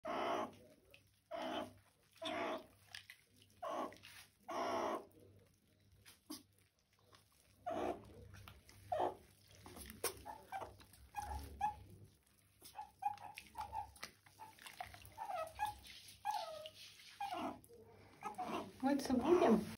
Meerkat Enjoying a Bath || ViralHog
Occurred on January 22, 2021 / Perm, Russia

Meerkat taking a bath.